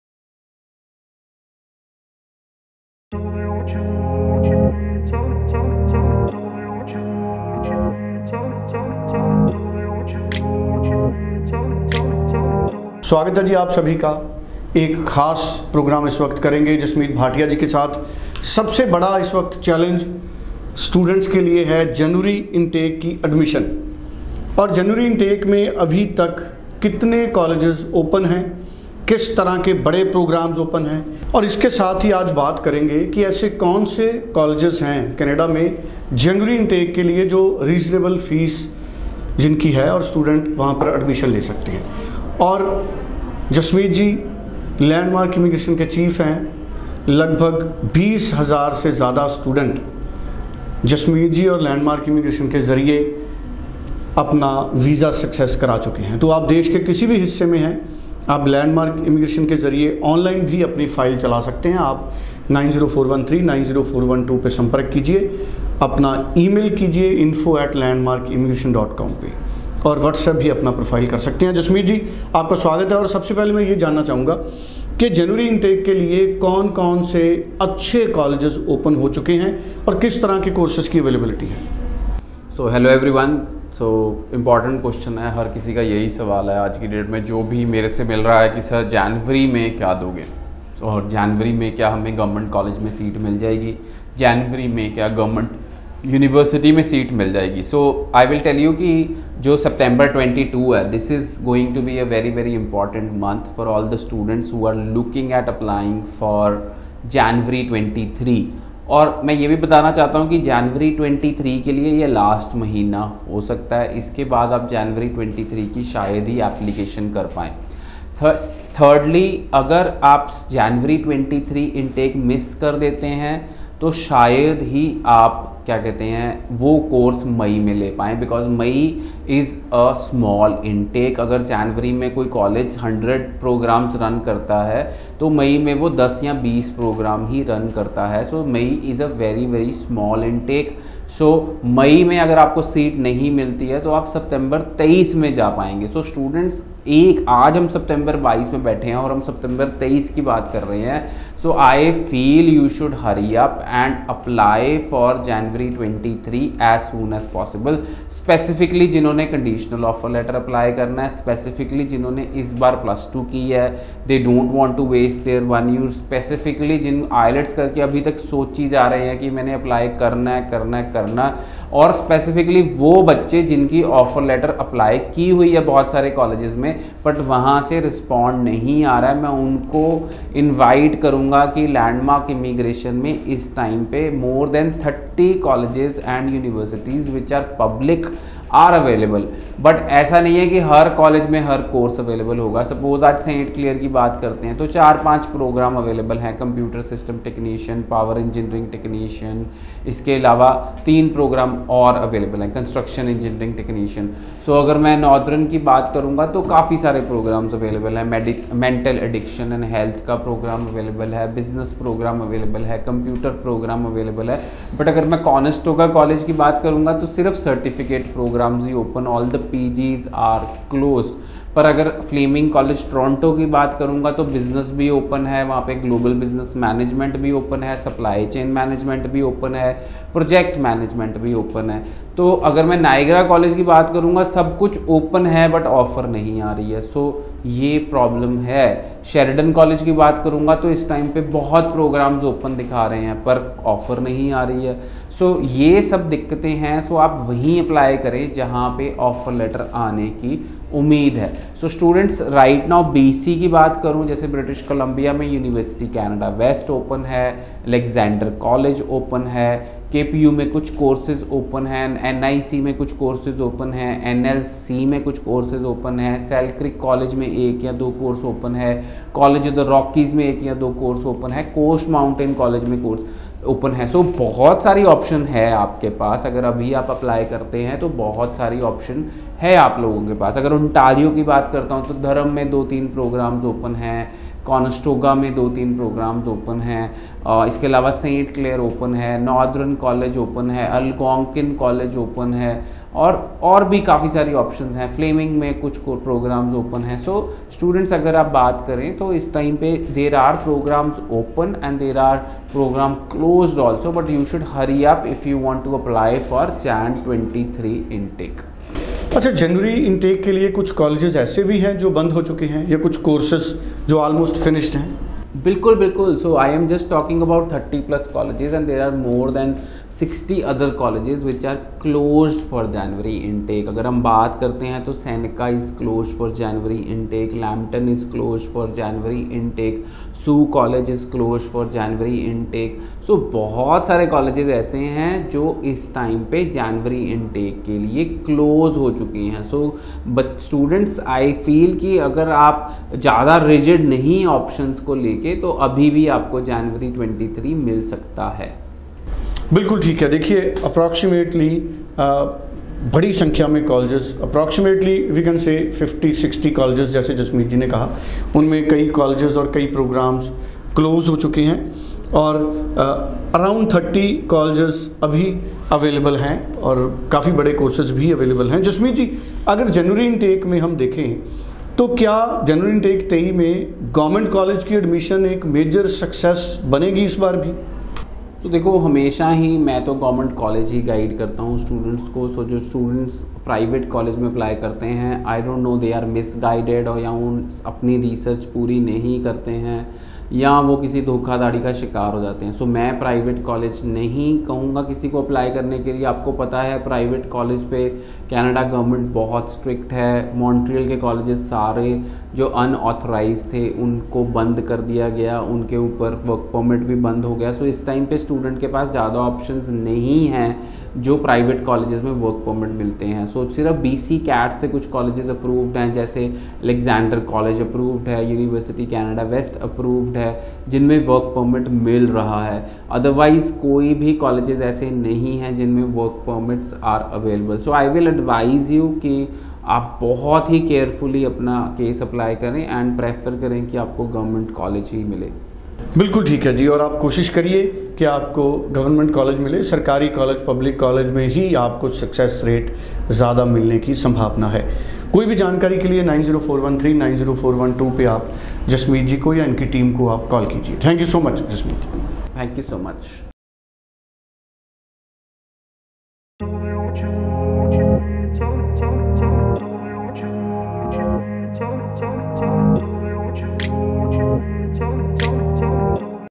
Canada Breaking News | Colleges Closed for Jan Intake 23 | Colleges Open for Jan 23
Canada Breaking News
Colleges Closed for Jan Intake 23
Colleges Open for jan 23.